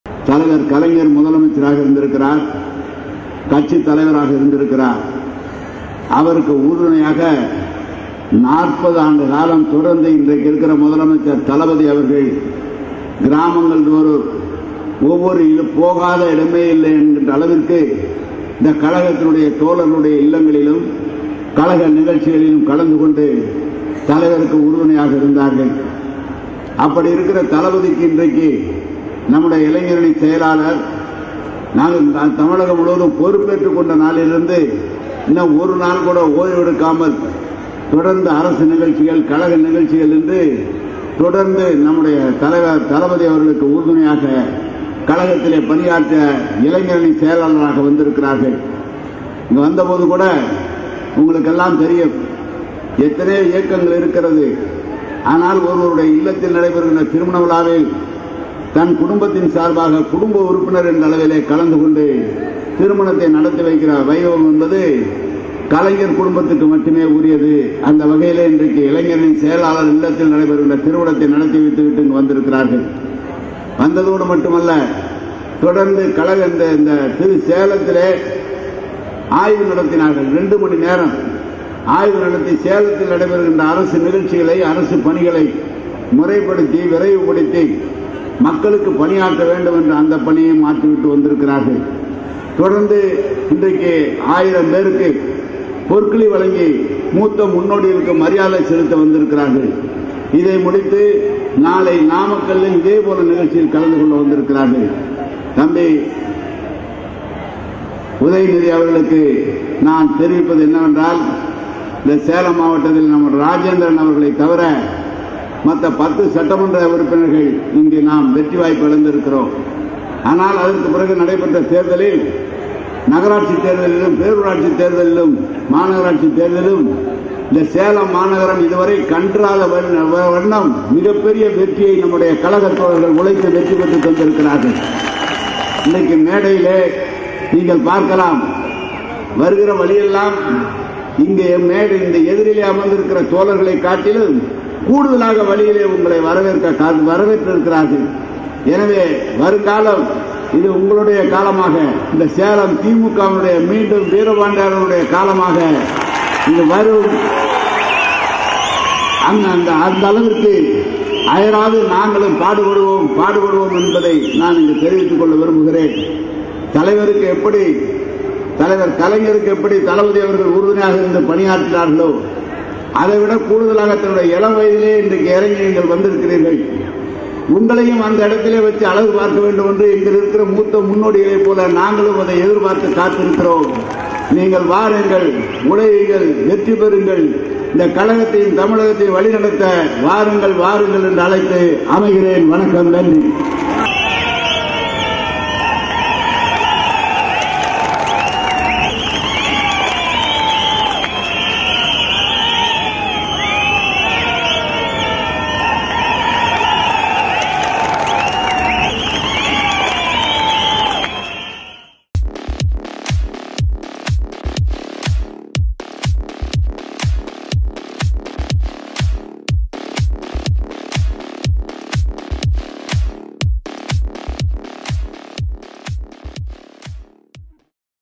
Mass காட்டிய KN நேரு | KN Nehru Mass Speech at Selam District Edappadi | Udhayanidhi | EPS
Mass காட்டிய KN நேரு | KN Nehru Mass Speech at Selam District Edappadi | Udhayanidhi | CM MK Stalin | EPS